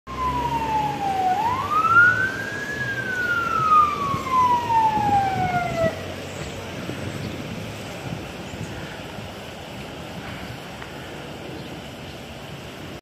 North Hudson Regional Fire And Rescue Reserve Battalion 4 Responding And Arriving In Kearny NJ 🚒🚒🚒
Reserve Battalion 4 2013 Ford Explorer ( Ex Battalion 2 )

Location Kearny NJ

Assignment 5th Alarm Fire